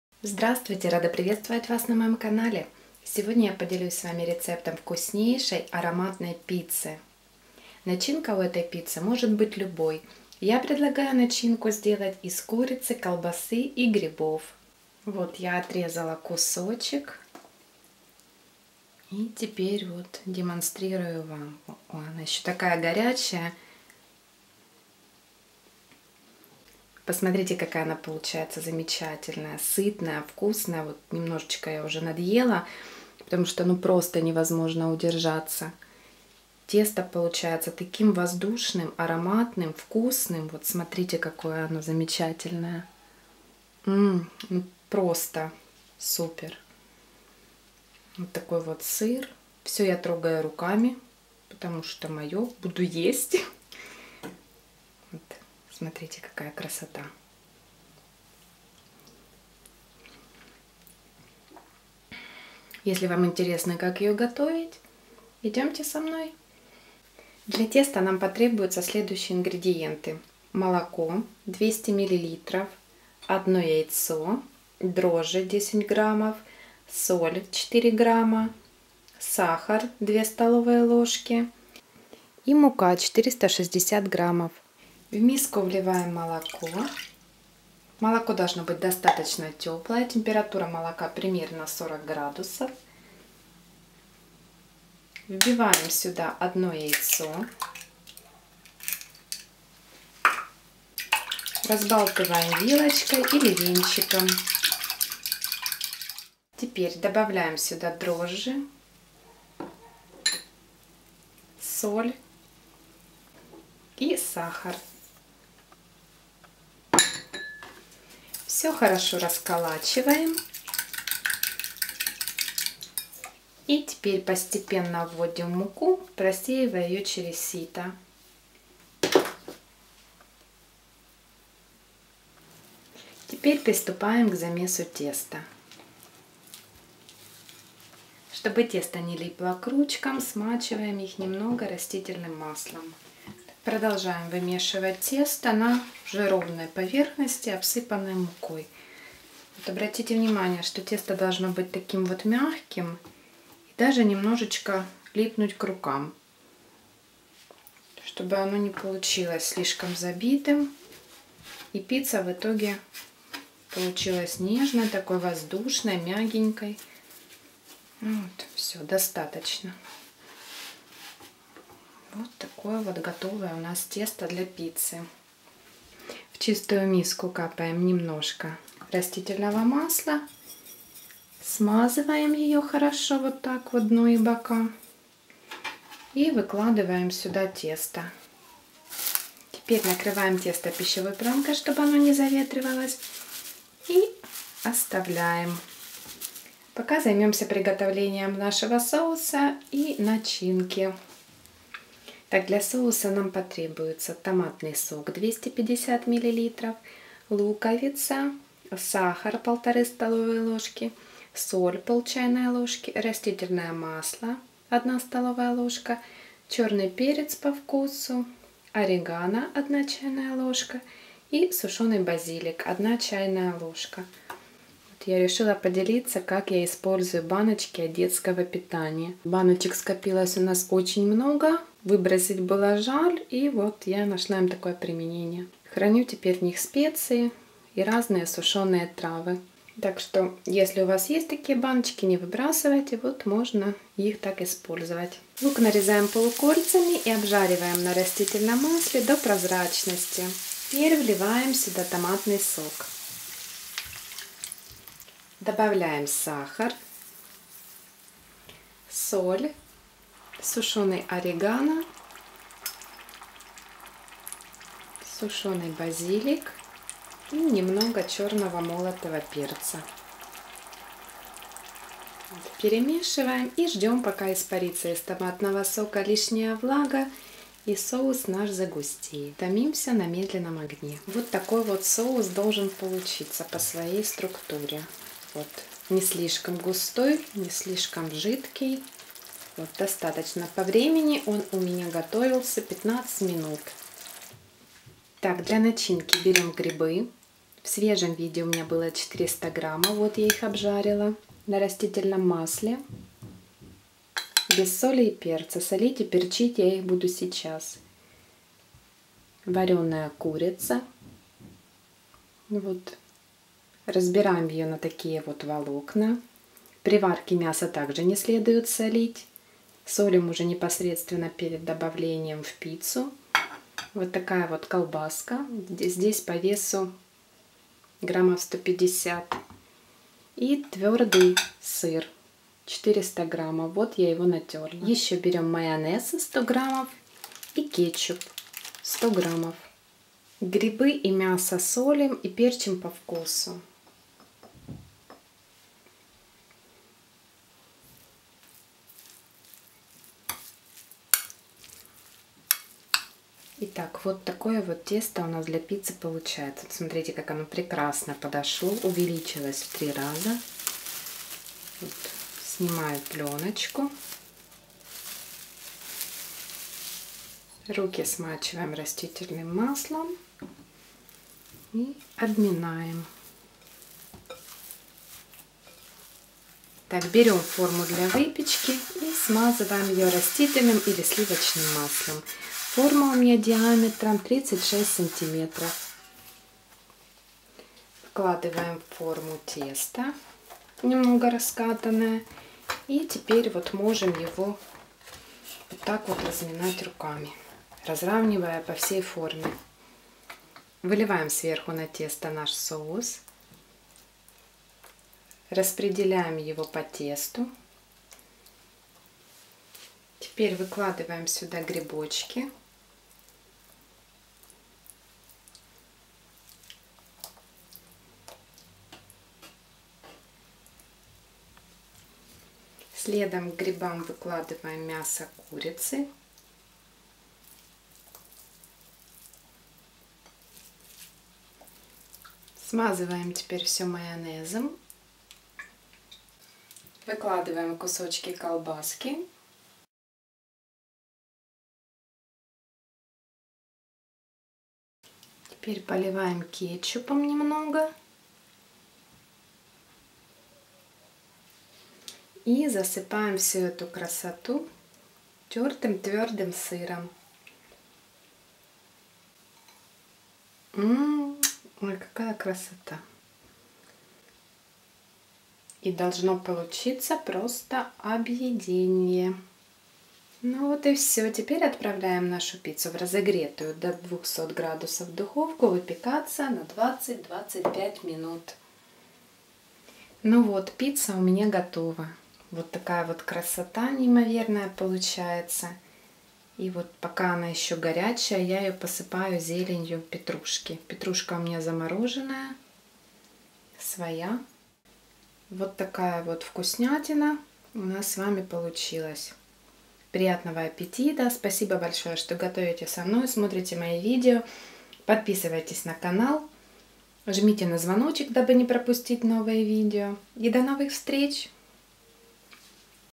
Пицца домашняя рецепт/Рецепт пиццы в духовке
В видео рецепт пиццы в духовке, я подробно рассказываю, как приготовить тесто, вкусный соус, начинку и сам процесс приготовления)))
Ингредиенты для теста:

Ингредиенты для соуса:
томатный сок 250 мл,
орегано 1 ч. л.,
базилик 1 ч. л.,
растительное масло 1 ст. л.
Начинка может быть любой!
У меня - это грибы жареные, мясо курицы отварное, колбаса, сыр и зелень петрушки
Форма для выпечки пицца в домашних условиях у меня диаметром 36 см
Выпекаем пиццу при 200 градусах 20-25 минут!